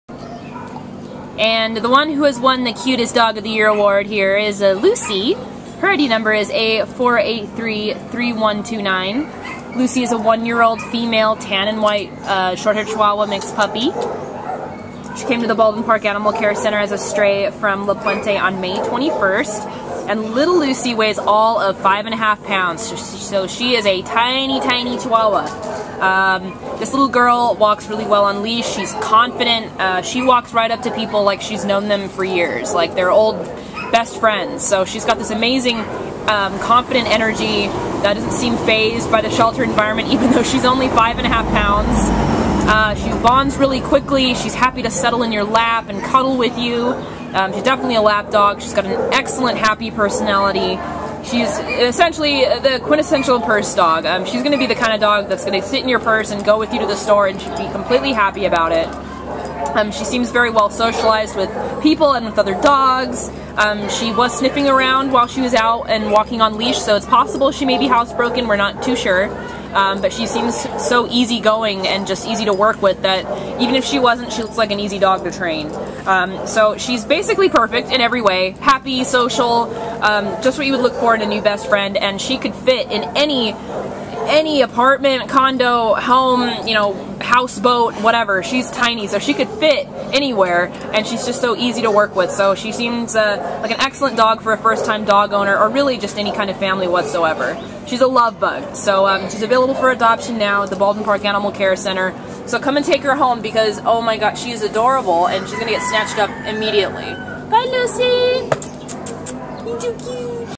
A4833129 Lucy | Chihuahua Mix
A4833129 Lucy is a joyous 1-year-old tan-and-white female Chihuahua mix puppy who was found in La Puente on May 21st and brought to the Baldwin Park Animal Care Center. Weighing 5 ½ lbs, Lucy is a perky girl with one of the happiest little lap/purse dog personalities around! Sweetly confident, Lucy walks right up to people like she’s known them for years. She also approaches other dogs happily and without hesitation, giving both her kennel mates and new canine friends kisses in greeting! Lucy walks well on leash and shows signs of being housebroken. We have seen her bond quickly with our handlers, and so we know that she will be a constant companion to whoever is lucky enough to call themselves her new forever family. With her brown eraser-looking nose and softly buggy eyes, Lucy will be an adorable and dear indoor pet for just about any household that can respect her small size.